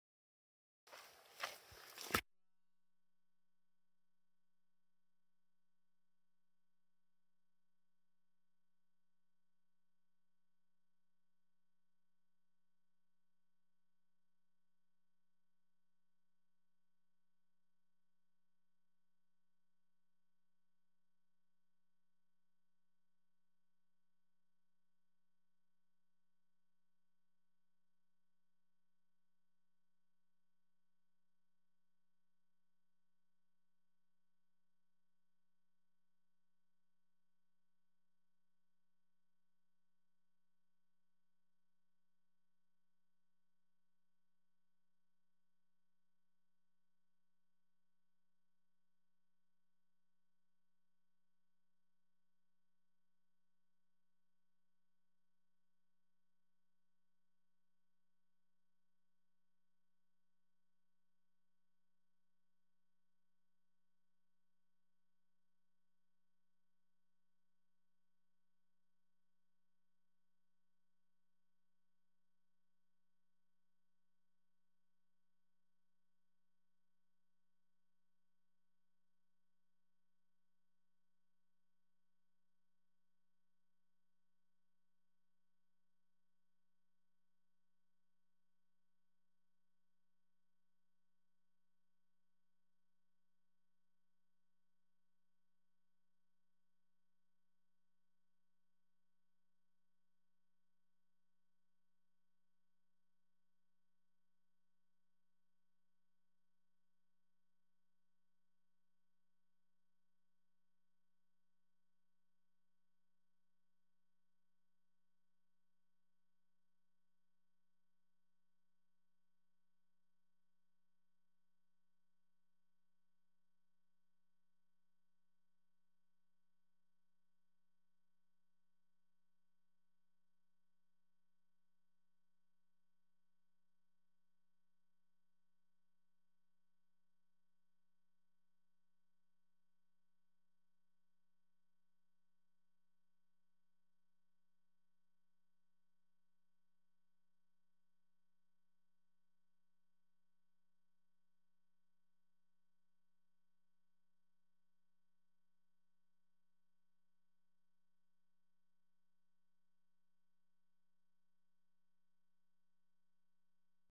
muddy road - rubber boots
walking on some really dirty tracktor track